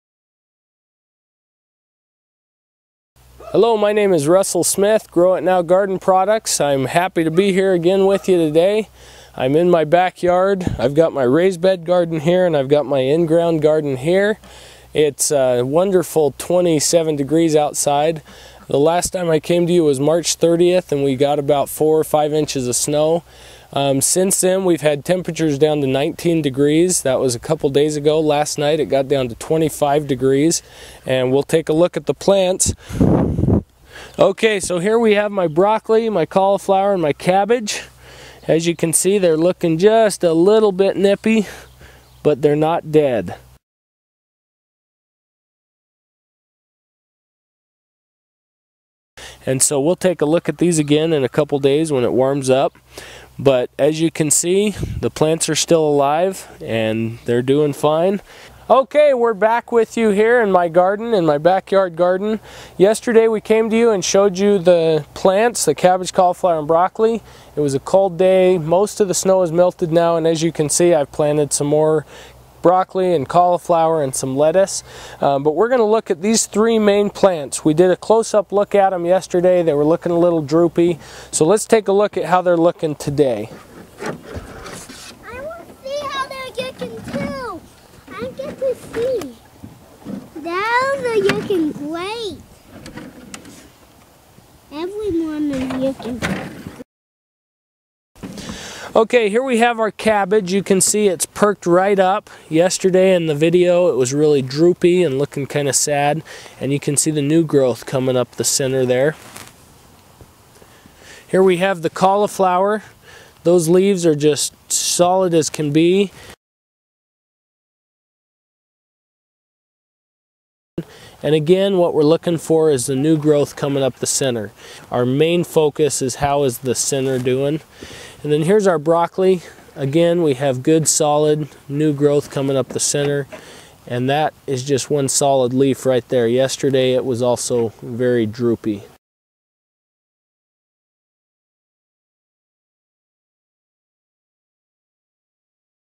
Plants under Mini Greenhouse after 5 days of cold
See how well the Grow it Now! Mini-Greenhouses Protect Plants from Cold Weather Temperatures.  In this video Segment you will see the plants at 8:30 am on a cold spring morning and again the next day when the sun is out.  Here you can see how well these mini-greenhouses work to help you start your plants early so you can extend your growing season.  for more great videos on planting using the Grow it Now! Products, Smart Beds and Mini-Greenhouses.  to purchase your Grow it Now! Products today.